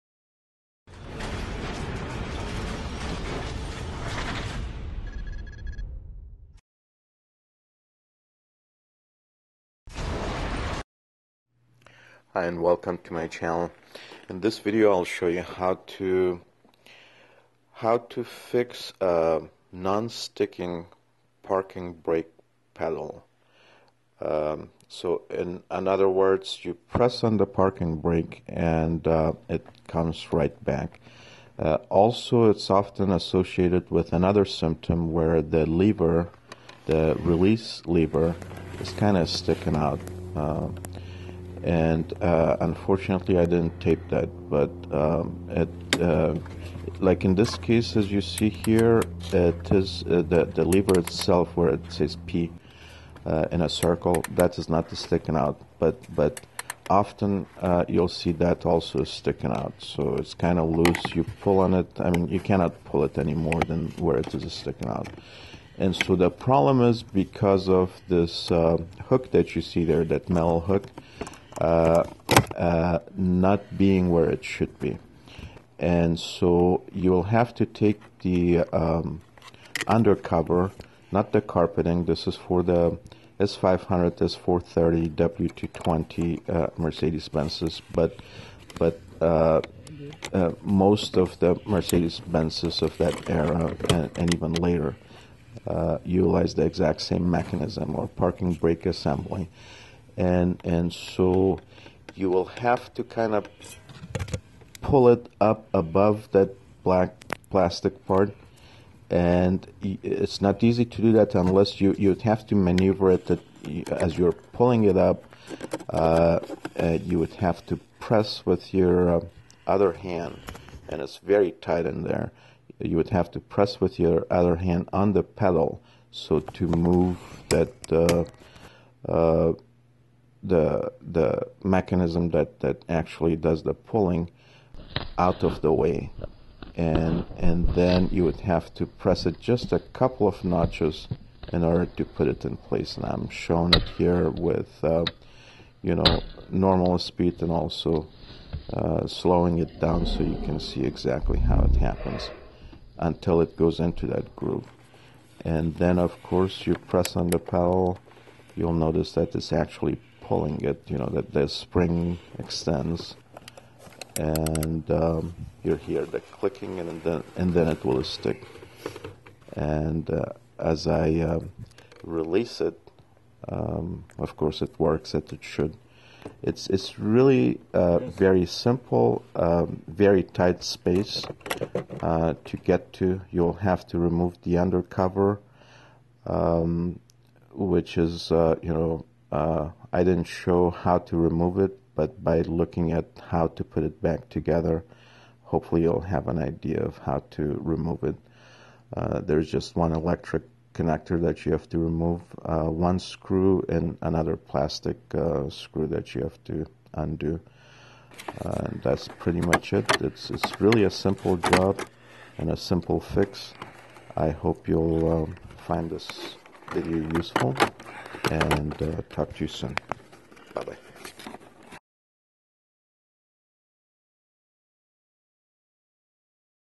Mercedes W220 S500 S430 Parking Emergency Brake Pedal Not Sticking
This is a simple fix where the latch or hook comes off loose.  Instead of a costly bill to replace the entire brake assembly, simply just move the latch back to its place.  This procedure applies to many Mercedes Benz models and is not limited to the W220.